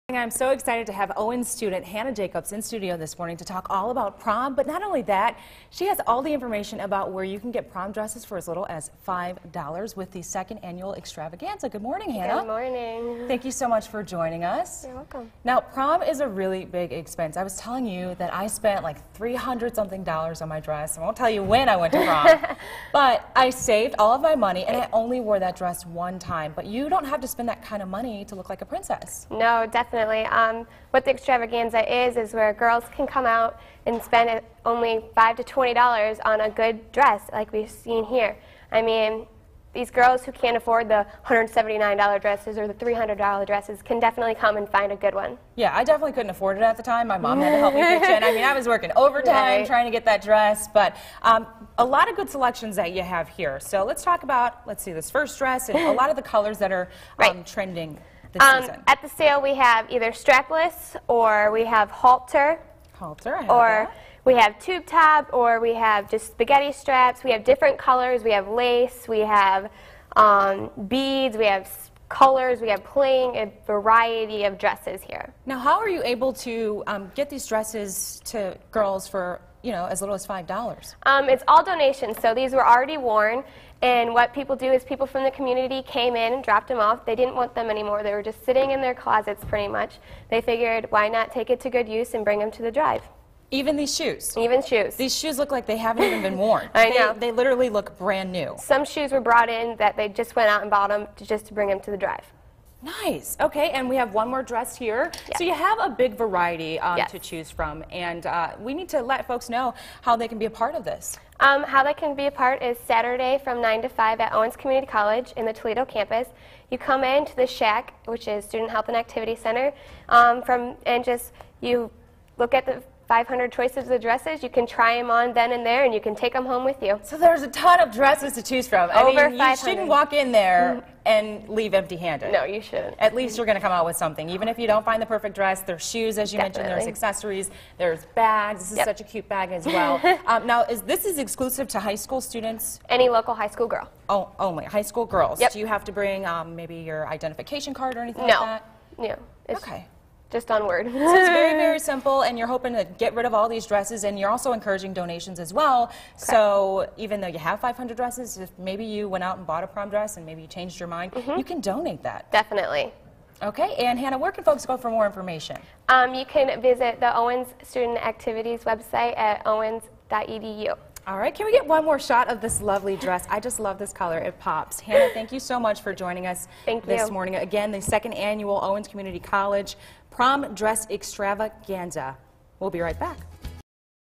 Get prom dresses cheap at Owens Extravaganza
Owens Community College will host its Prom Dress Extravaganza on Saturday, April 13